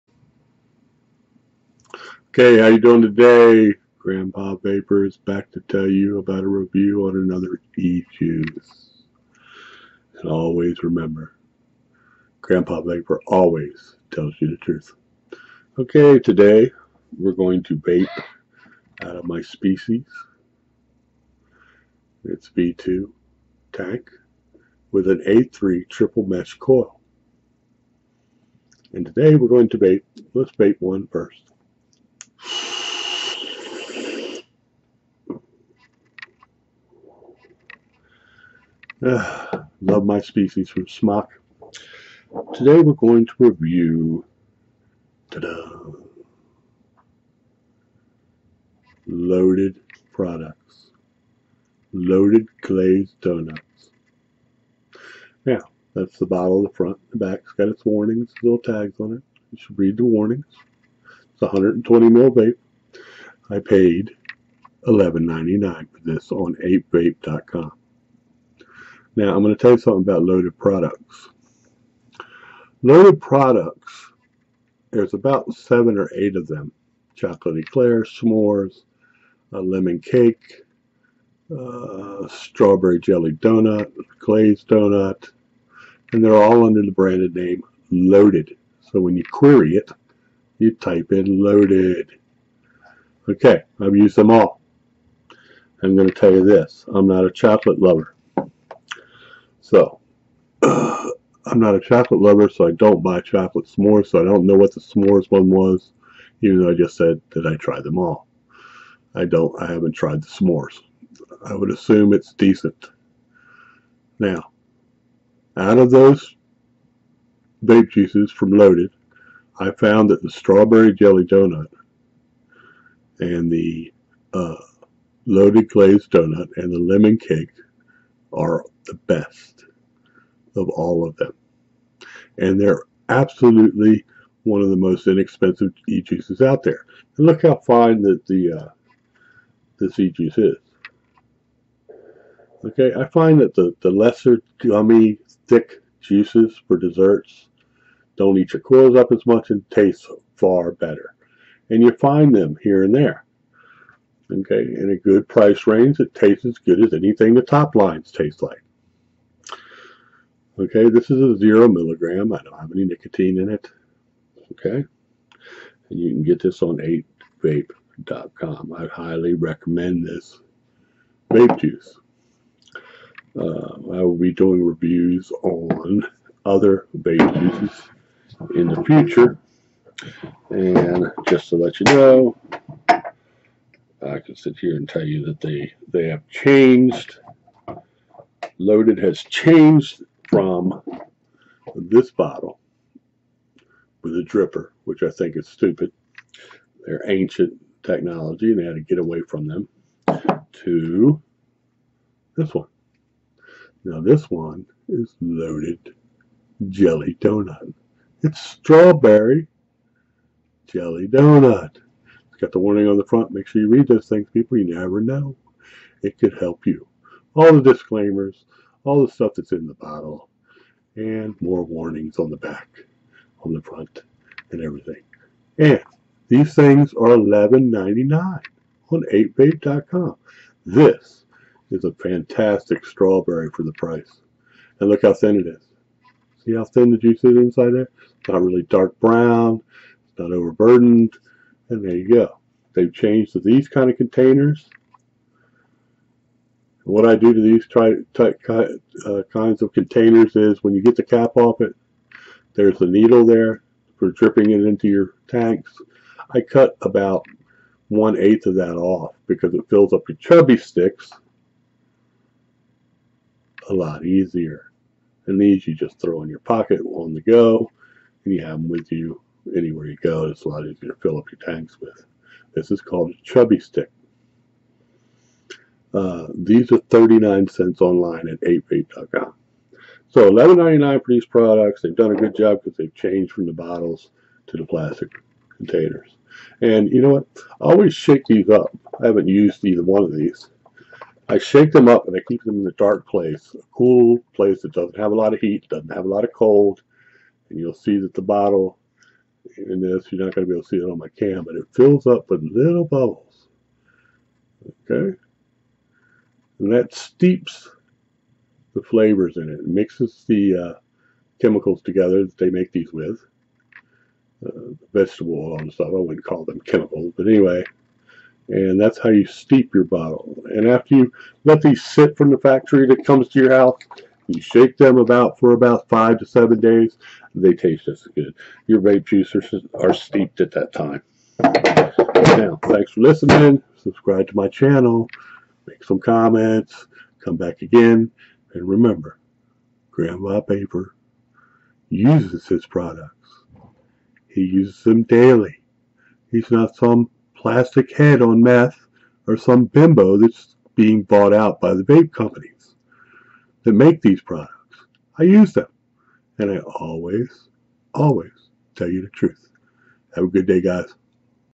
LOADED E JUICE GREAT LOW COST JUICE
GRANDPA VAPOR , LOADED E JUICE GREAT LOW COST JUICE. THIS IS ONE OF THE BEST BRANDS OF LOW COST E JUICE AND 120 MILS. OUTSTANDING BUY ..I FOUND IT ON EIGHTVAPE.COM FOR 11.99 US DOLS.